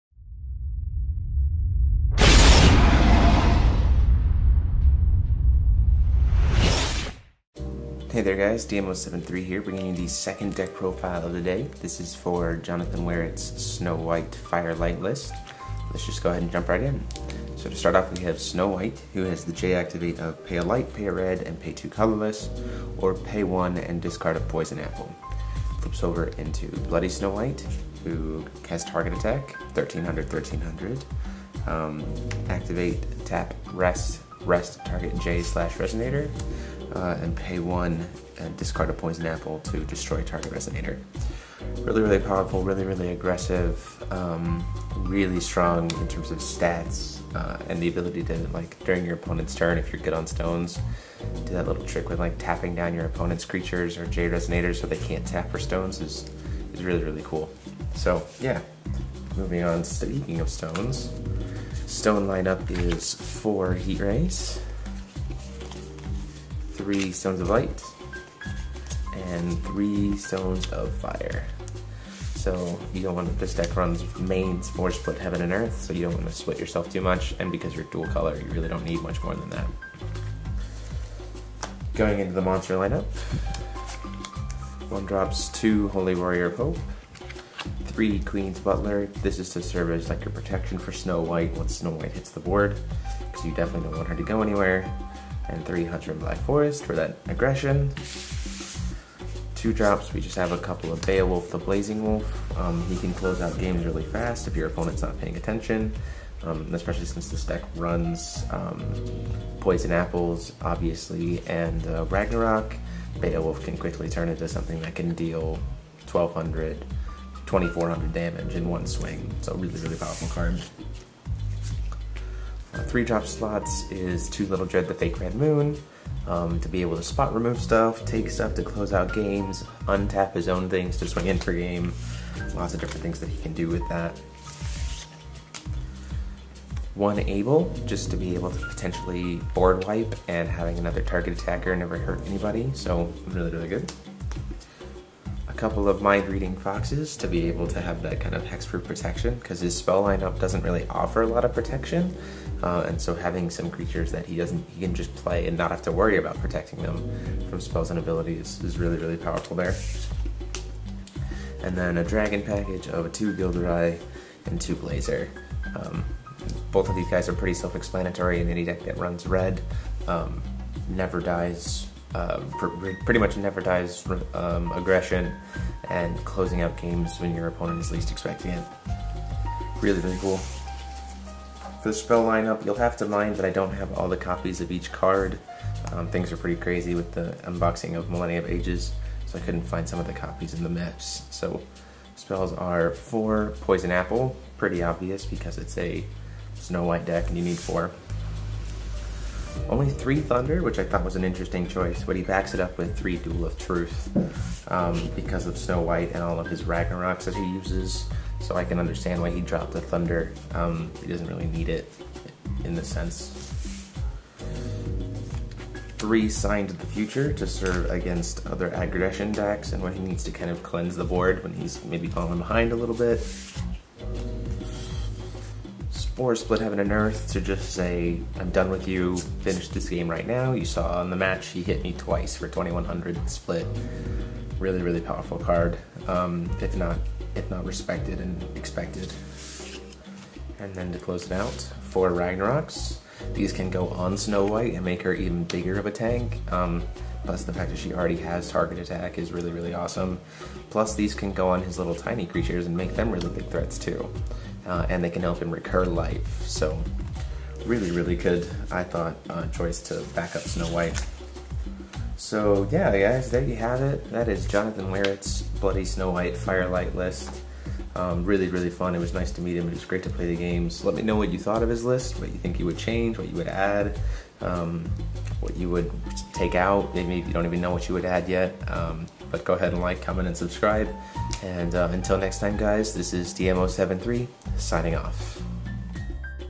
Force of Will (TCG) Deck Profile: Fire/Light Snow White
Hey there guys! Here is the list for the Jonathan Wherrit's Fire Light Snow White Deck!

Ruler:

Snow White/Bloody Snow White

Magic Stones:

4 Magic Stone of Heat Ray
3 Magic Stone of Flame
3 Magic Stone of Light

Resonators:

3 Hunter in Black Forest
3 Queen’s Butler
2 Holy Warrior of Hope
2 Beowulf, the Blazing Wolf
2 Little Dread, the Fake Red Moon
1 Abel, the Avenger of Gods
2 Mind Reading Fox
2 Blazer the Eater of Dimensions
2 Gille de Rais, the Golden Dragon

Spells:

4 Poison Apple
3 Thunder
3 Duel of Truth
3 Sign to the Future
4 Split Heaven and Earth
4 Ragnarok, the Divine Sword of Savior

Video Opener
"RavenProDesign"